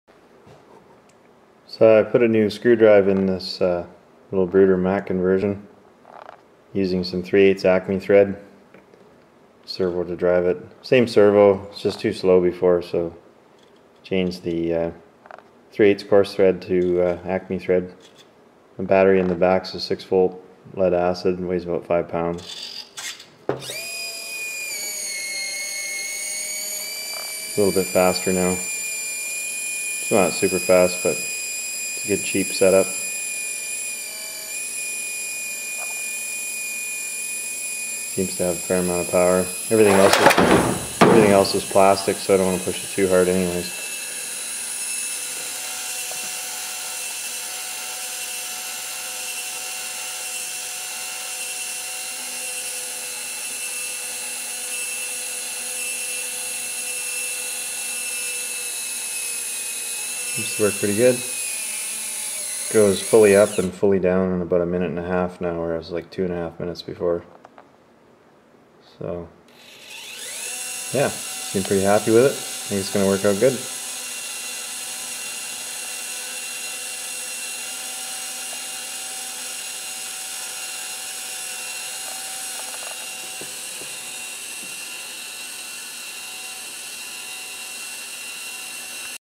Upgraded screw drive in the Bruder Mack
Updated to ACME thread from NC to increase dump speed.